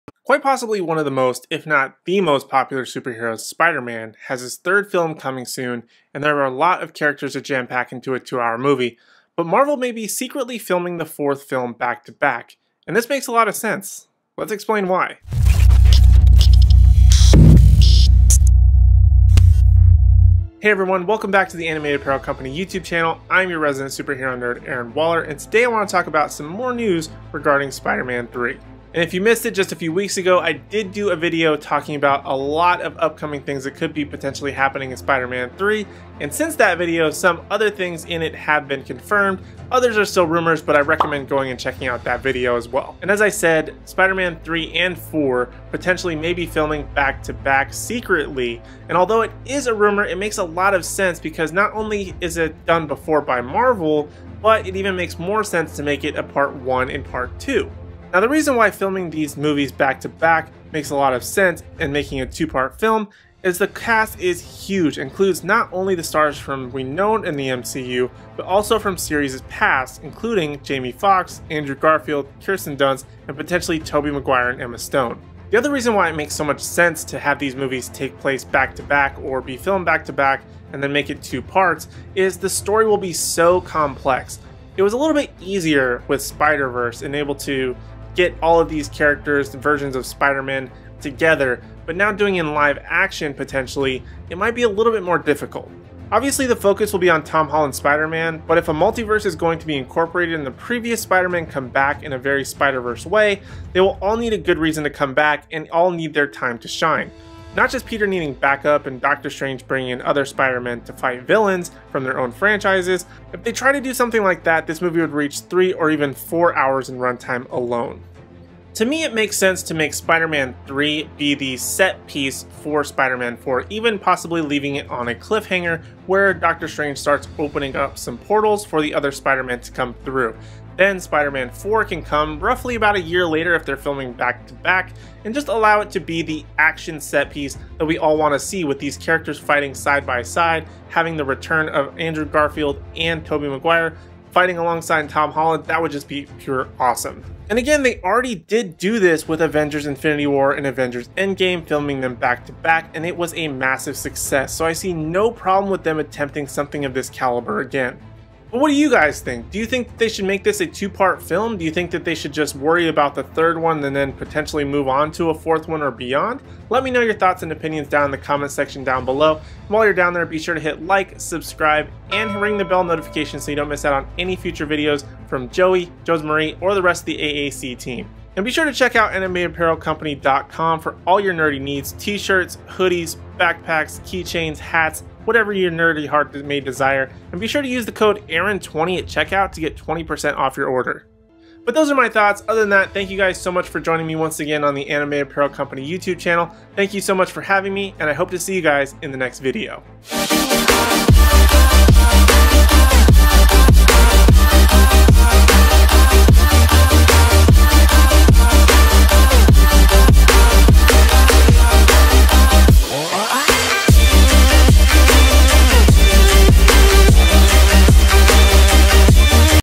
Spider-Man No Way Home Rumors | Spider-Man No Way Home Movie Updates | Spider man No Way Home Leak
It has been rumored that Spider-Man 3 and 4 may be secretly filming back to back. Marvel has done this before and it may make perfect sense considering the number of characters and the story it will contain. Are you excited for this epic Spider-Man collab? Let us know what you think of the recent Spider-Man 3 & 4 rumors and updates in the comments!

FOLLOW AAC on Social Media and JOIN THE COMMUNITY